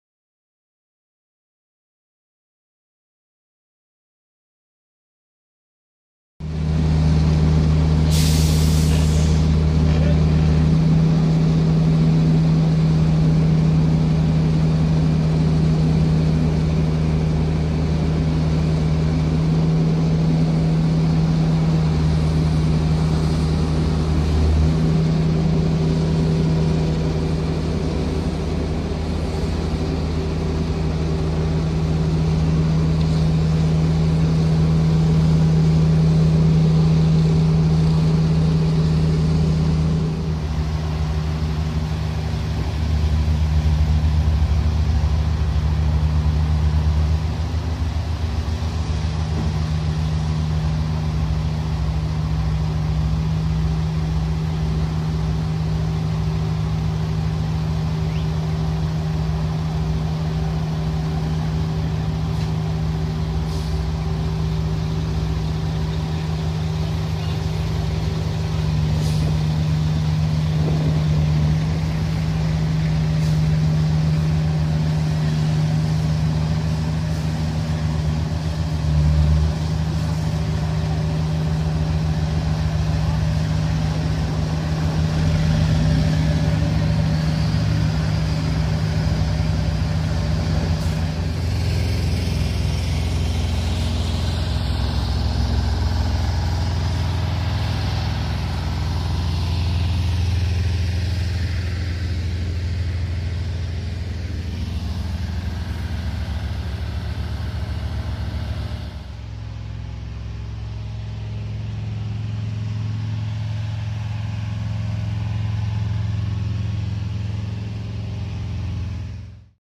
Paving Driveway - November 23rd, 2013
Final paving of the driveway we've been working on in my other recent videos.